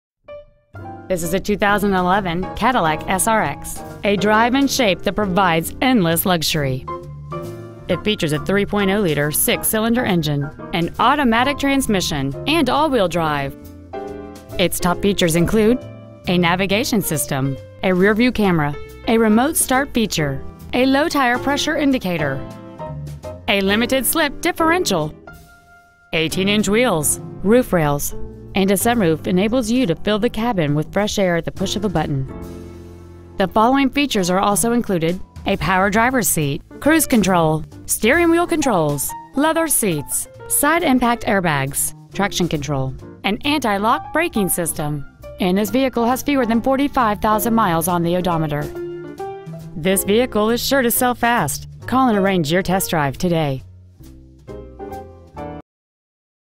2011 Cadillac SRX Chicago IL
Year : 2011
 Make : Cadillac
 Model : SRX
 Engine : 3.0L VVT DOHC V6 SIDI Engine
 Trans . : Automatic

 Miles : 44,808

 115 W South Frontage Rd.

 Preowned 2011 Cadillac SRX  Bolingbrook IL